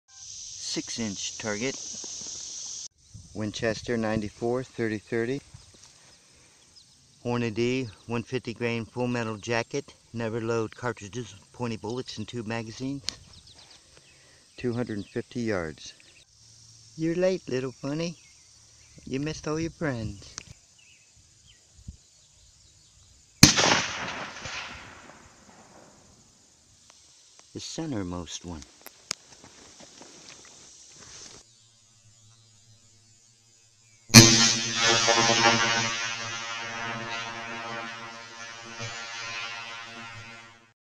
Center Hit
Taking a shot with he Winchester 94 30-30, at a 6 inch target, 250 yards downrange; hit!

It took me long enough.